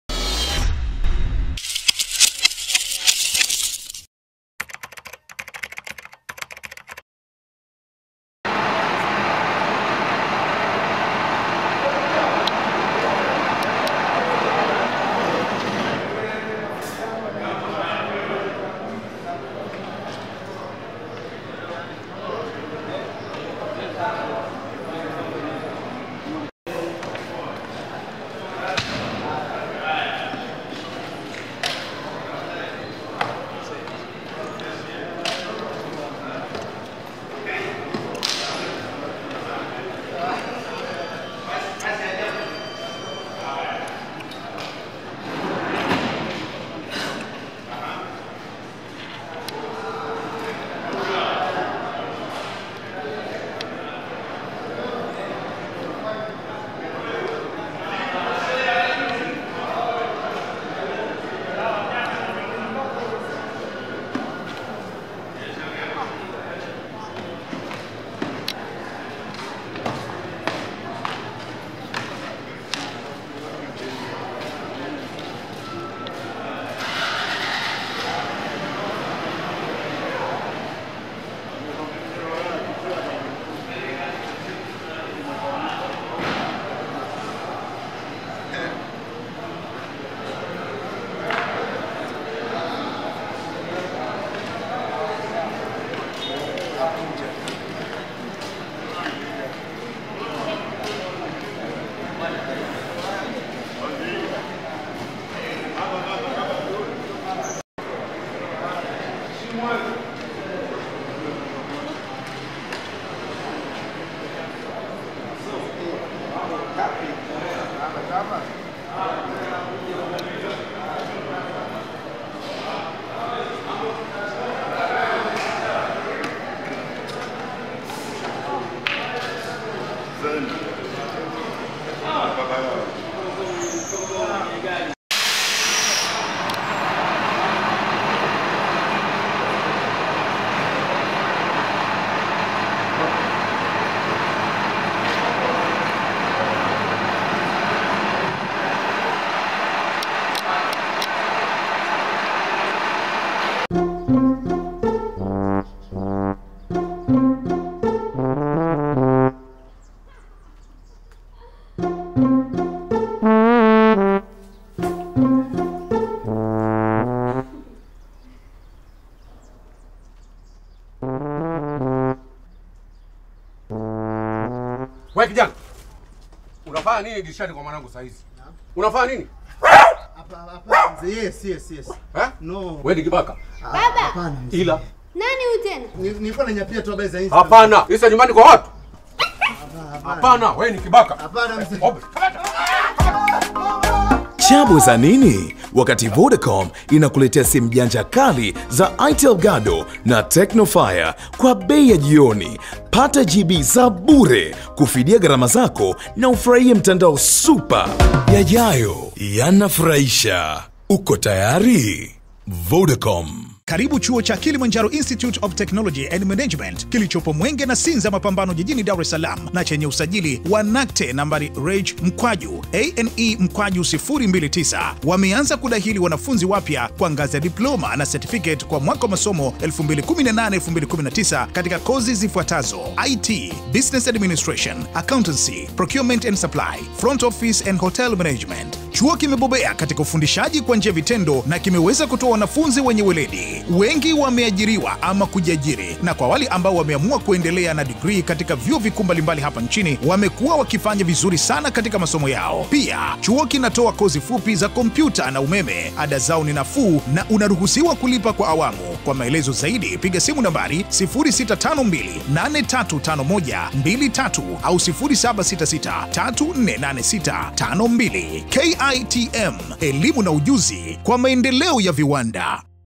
SIMBA Walivyoingia Taifa Kuiua Mbeya City (SIMBA VS MBEYA CITY)
KIKOSI cha Simba kimeshuka katika uwanja wa Taifa jijini dar es Salaam kukipiga na Mbeya City ya jijini mbeya ikiwa ni muendelezo wa Ligi Kuu Tanzania Bara.

Simba katika mechi ya ufunguzi ilikutana na Tanzania Prisons na kuibuka na ushindi wa bao 1-0 mechi iliyofanyika katika uwanja wa Taifa jijini Dar es salaam.